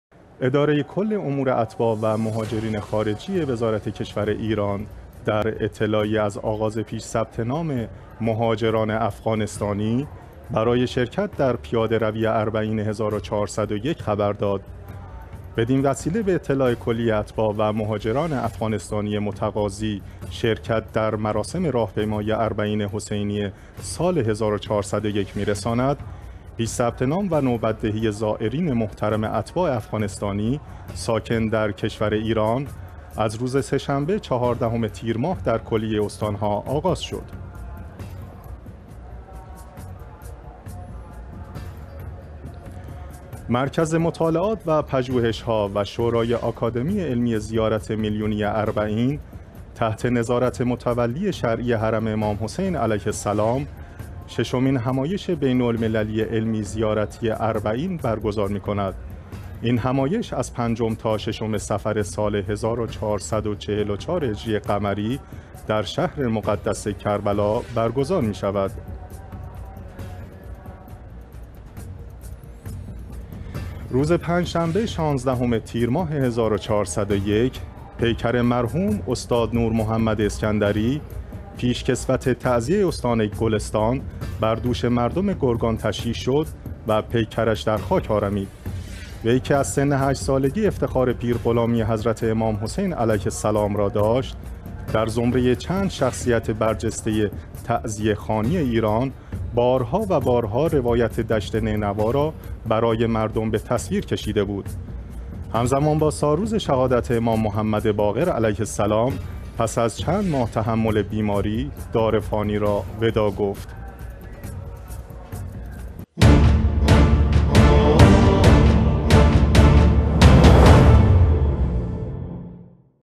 نگاهی به چند خبر کوتاه: 18 تير 1401
1)آغاز ثبت نام مهاجران افغانستانی در ایران برای شرکت در اربعین ۱۴۰۱
2)آخرین مهلت ارسال مقالات به همایش بین‌المللی اربعین
3)تشییع و تدفین پیکر پیشکسوت تعزیه خوانی استان گلستانِ ایران